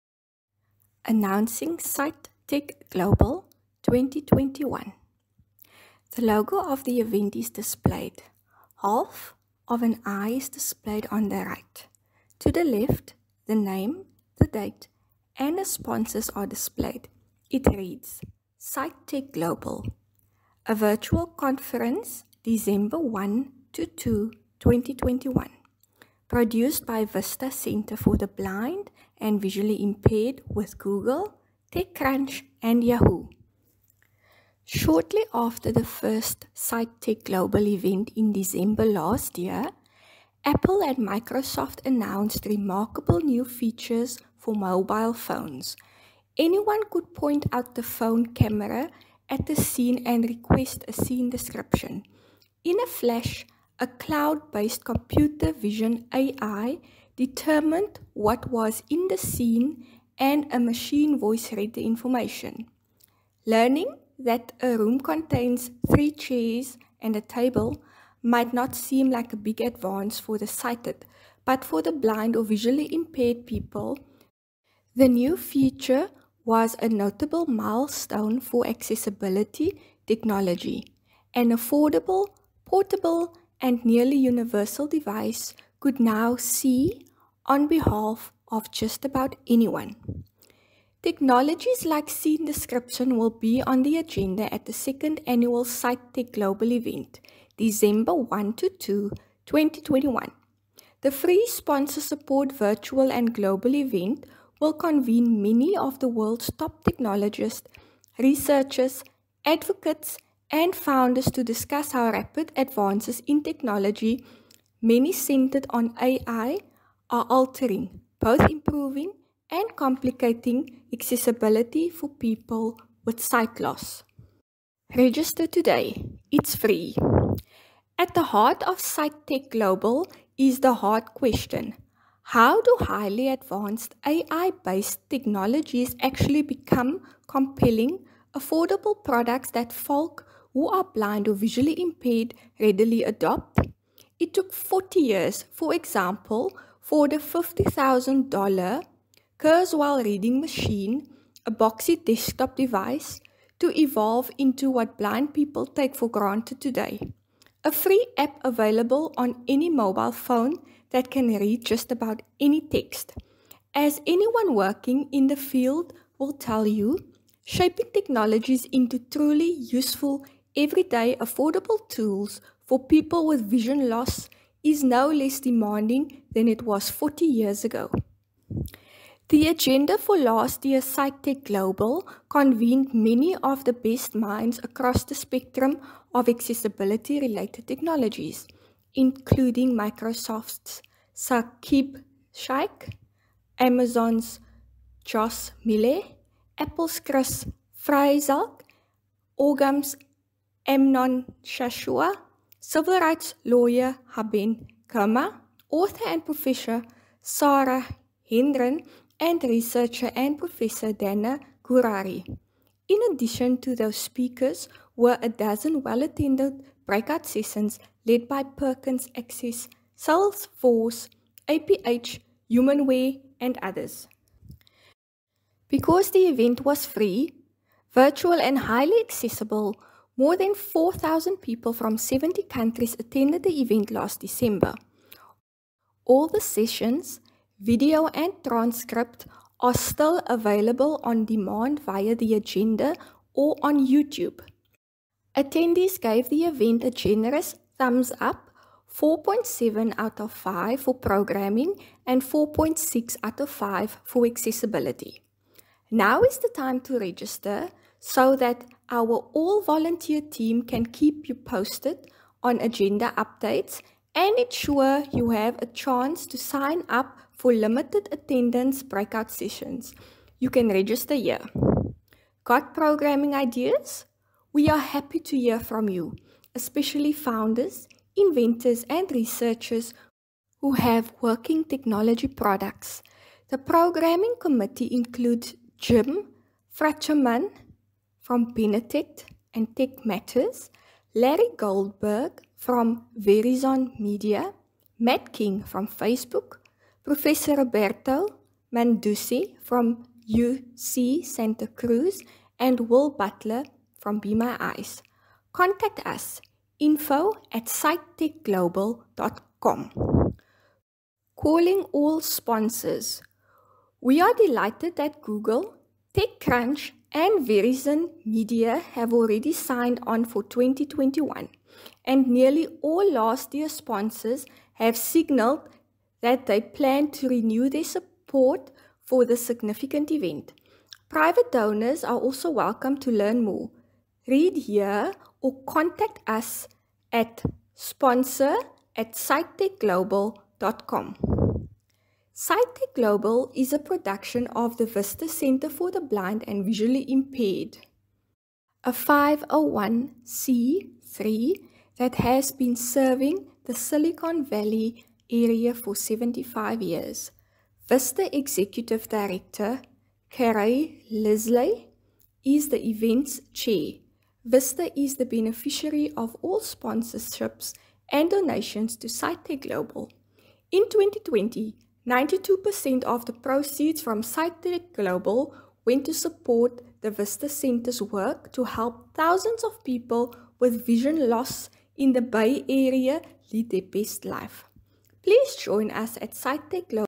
The Torch 20: Sight Tech Global
Shortly after the first  Sight Tech Global event, in December last year, Apple and Microsoft announced remarkable new features for mobile phones. Anyone could point the phone camera at a scene and request a “scene description.” In a flash, a cloud-based, computer vision AI determined what was in the scene and a machine-voice read the information.